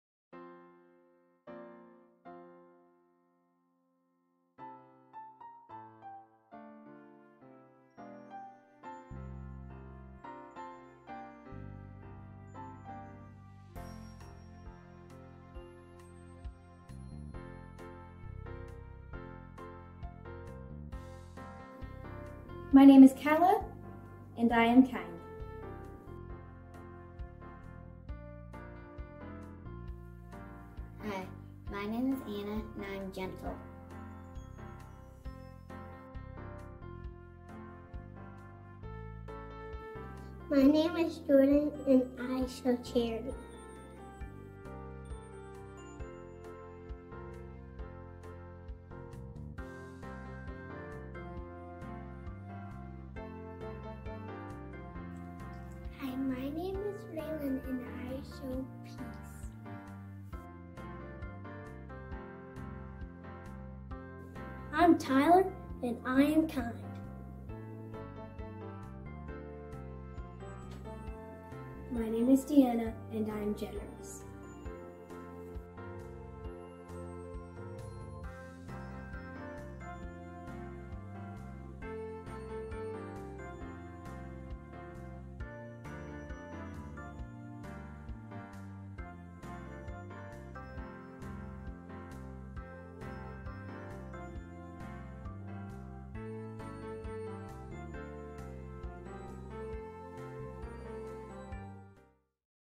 Norwalk Catholic Fruit of the Spirit Elementary Students
Six students share how they embody one of the fruits of the spirit.